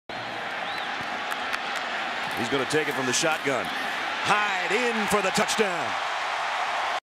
Carlos Hyde 1 Yard Touchdown
Ohio State's Carlos Hyde rushes for a 1 yard touchdown in the second quarter against Nebraska.